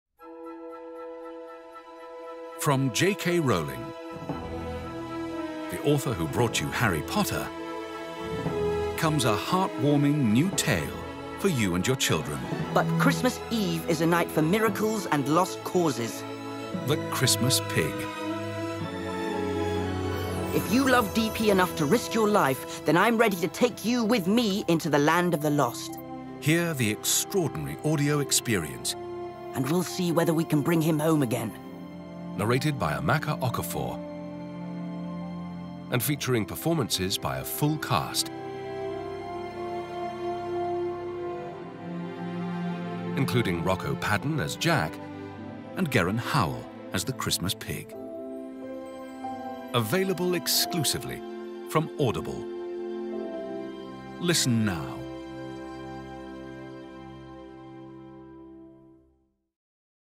Christmas Pig | Official Trailer | Audible India
From the author of Harry Potter comes another holiday adventure full of warmth and joy! One boy and his toy are about to change everything! To find out how? Listen to, ‘The Christmas Pig’ by J.K. Rowling now!